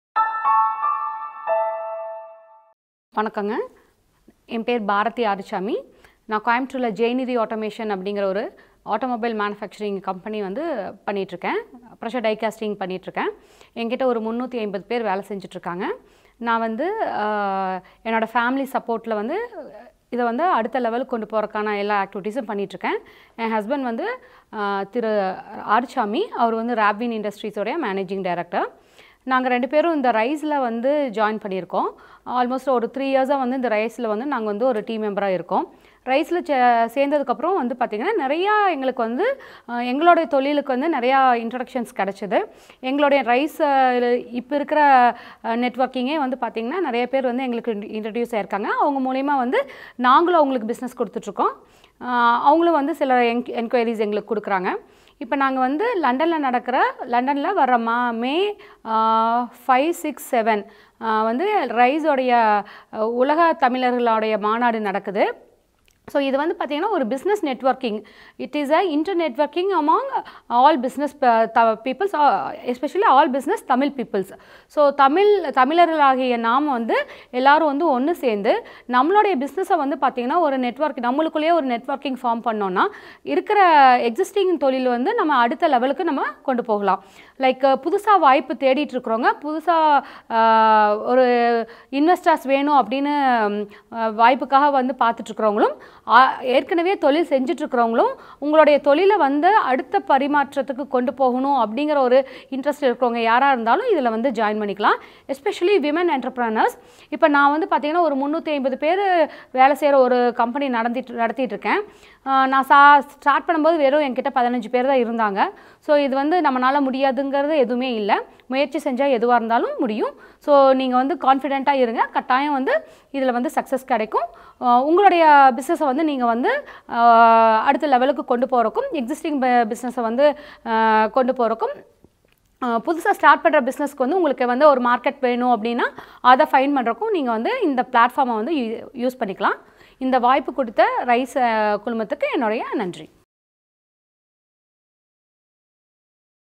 உங்கள் தொழிலை அடுத்த பரிமாணத்துக்கு எடுத்துச் செல்ல லண்டனுக்கு வாருங்கள்!!!!
The Rise அமைப்பில் சேர்ந்த பிறகு எங்களுக்கு வணிக தொடர்புகள் அதிகம் கிடைத்தன. இந்த அமைப்பு லண்டனில் ஒருங்கிணைக்கும் மாநாட்டில் நீங்கள் கலந்துகொண்டால் உலகளவில் உங்களுக்கு வணிகத் தொடர்புகள் கிடைக்க வாய்ப்புகள் ஏற்படும் அதன் மூலம் உங்கள் தொழிலை விரிவாக்கலாம்

The Rise Emerge London Summit plans to create a remarkable stage for global Tamil entrepreneurs.
✅ Participants from 70+ Countries
✅ 40+ B2B Events
✅ Co-Market your products globally
✅ Meet unique Tamil Achievers
✅ Build a strong networking group
To Register